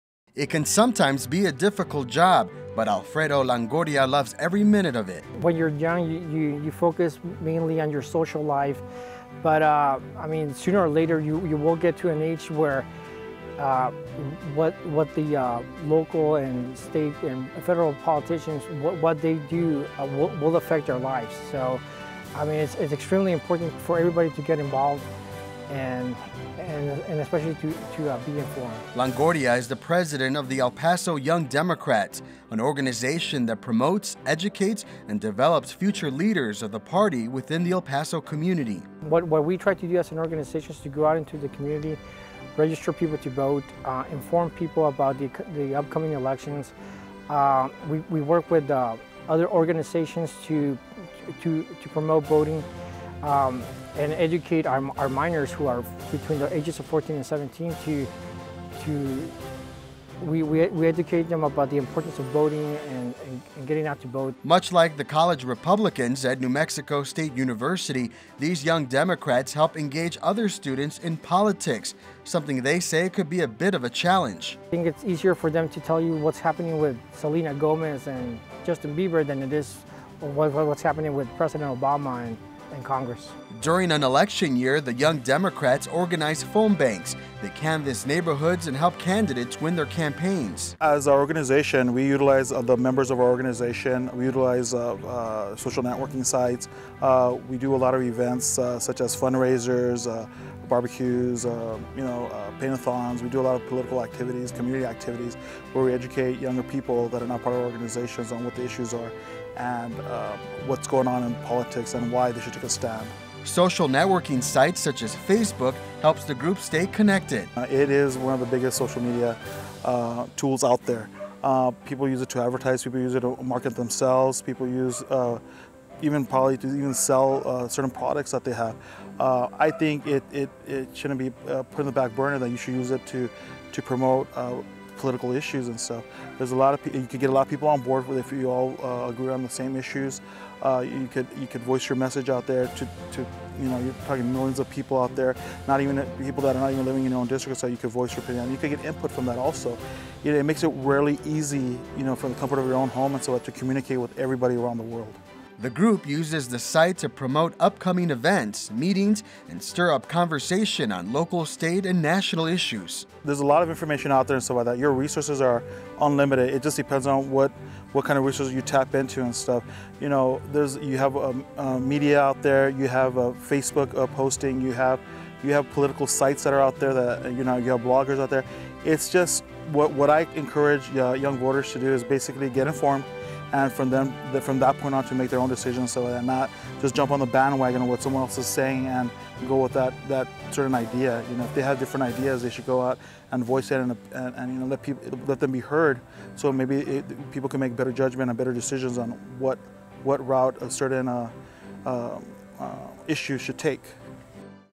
El Paso Young Democrats
09.20.11 (EL PASO) -- It can sometimes be a difficult job, but Alfredo Longoria loves every minute of it.

"When you're young you just mainly focus on your social life, but sooner or later you'll get to an age where what the local and state and federal politicians what they do will affect their lives so I mean, it's extremely important for everybody to get involved and especially to be informed," he said.

Longoria is the president of the El Paso Young Democrats, an organization that promotes, educates and develops future leaders of the party within the El Paso community.

"What we try to do as an organization is try to go out into the community, register people to vote, inform people about the upcoming elections -- we work with other organizations to promote voting and educate," he said.

Much like the college republicans at New Mexico State University, these young democrats help engage other students in politics, which they say can be a bit of a challenge.

"We utilize the members of our organization, we utilize social networking sites, we do a lot of events such as fundraises, barbeques, we do a lot of political activities, community activities where we educate younger people about our organization on what the issues are and what's going on in politics and why they should take a stand," he said.

During an election year, the young democrats organize phone banks, they canvass neighborhoods and help candidates win their campaigns.

"There's a lot of information out there.  You're resources are unlimited.  It just depends on what kind of resources you tap into and stuff, you have media out there, you have Facebook posting, you have political sites out there, blogging.  What I encourage younger voters to do is get informed and from that point to make their own decisions so they're not just jumping on the band wagon with what someone else is saying and go with that certain idea," he said.

Reported by Carlos Correa